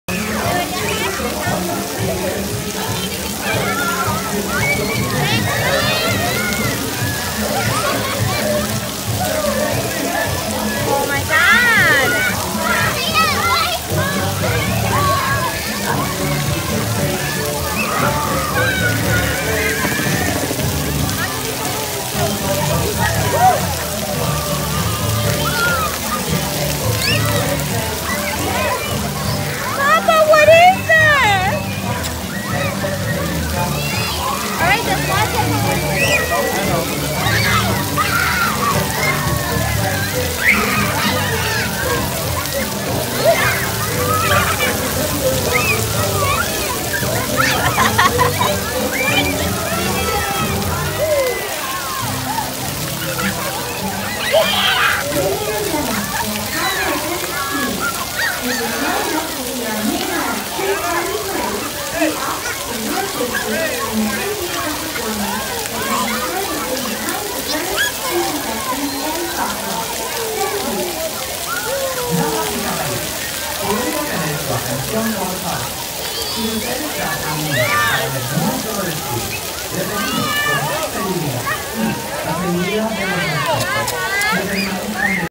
Seasame Street Splash Park with Eli
My wife was recording our son's first visit to the new Seasame Street splash park in Seaworld Orlando. We had a blast on this trip! Even though this clip is short Eli enjoyed his first trip to the splash park & we will definitely be back! Hope you enjoy this clip comment your favorite moments & share your experiences if you've been & to all families I recommend checking out the new Seasame Street world in Seaworld Orlando.